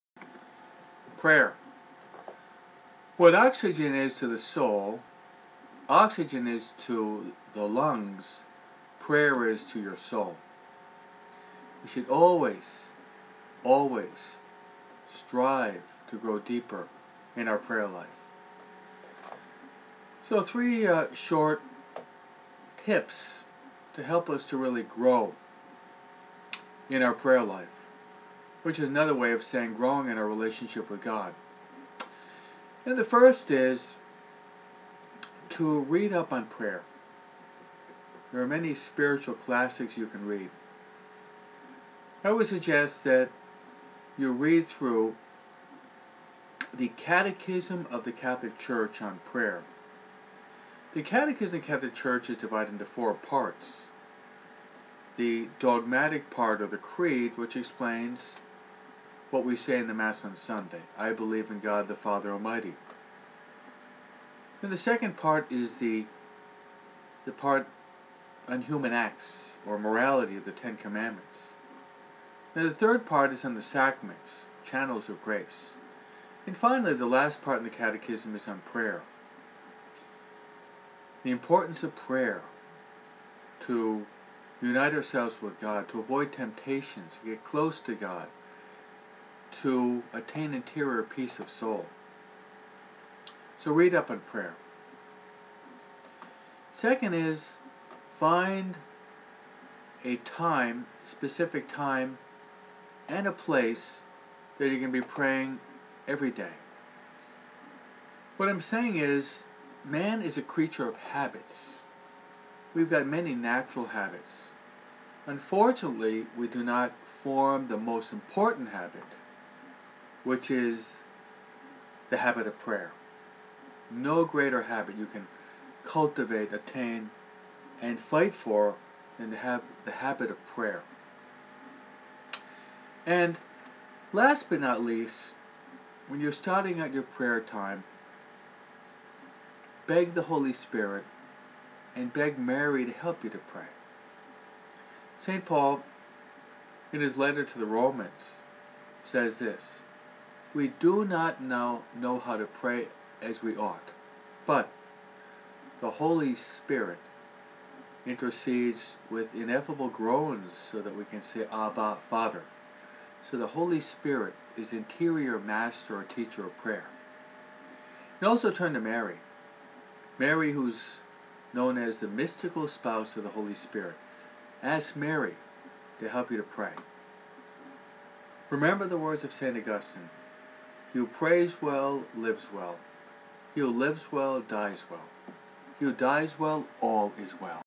PRAYER
Here are three tips to improve your prayer life!!!